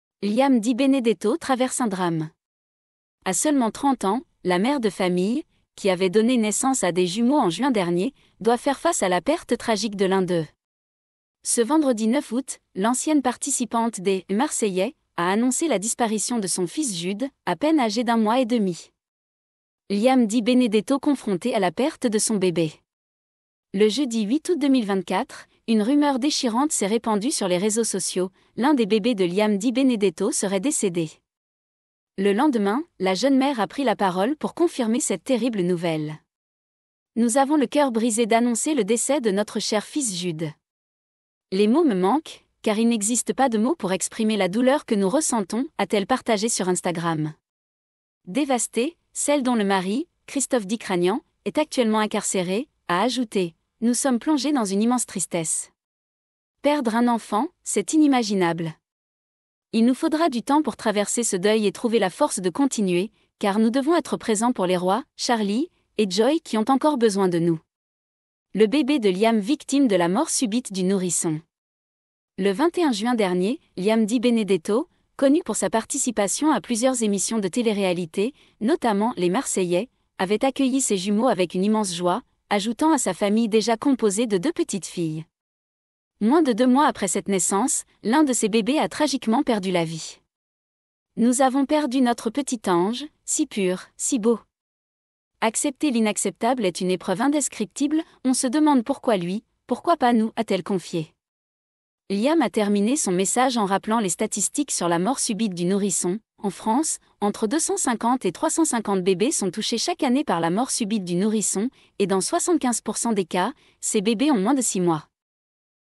Liam Di Benedetto Annonce la Tragique Perte de son Fils : 'Nous Avons Perdu Notre Petit Ange'
Liam Di Benedetto est en deuil. La maman de 30 ans, qui avait donné naissance à des jumeaux en juin dernier, doit aujourd'hui affronter la perte de l'un d'eux.

Ce vendredi 9 août, l'ancienne candidate des *Marseillais* a annoncé la mort de son fils Jude, survenue seulement un mois et demi après sa naissance. Le lendemain, elle a confirmé cette tragique nouvelle.